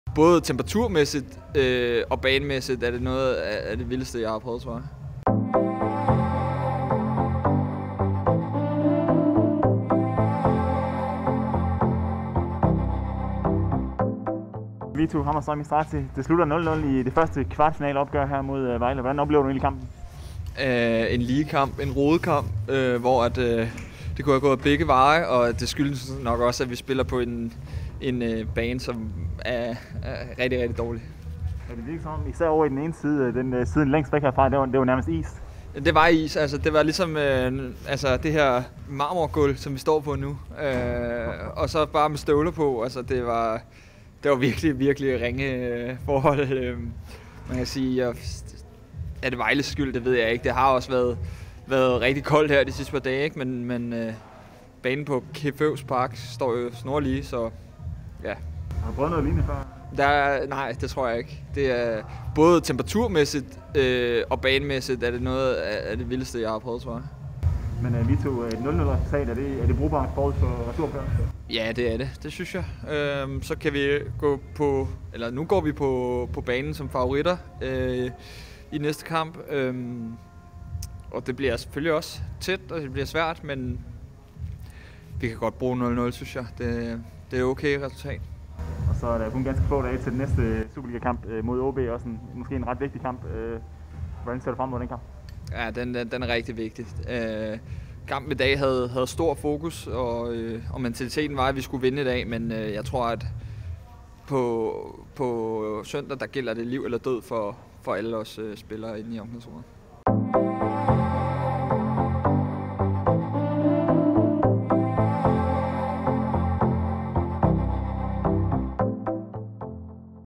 Vito: Noget af det vildeste jeg har prøvet
- Både temperaturmæssigt og banemæssigt er det noget af det vildeste, jeg har prøvet.

Vito Hammershøy-Mistrati var ikke just begejstret for aftenens "is-fodboldbane" i Vejle. Banen i Vejle har intet på Cepheus Park Randers, som ifølge midtbanekreatøren står snor lige.

0-0 resultatet i den første kvartfinale er brugbart, mener Vito, der forudser, at Randers FC går på banen som favoritter i returopgøret i Randers den 10. marts.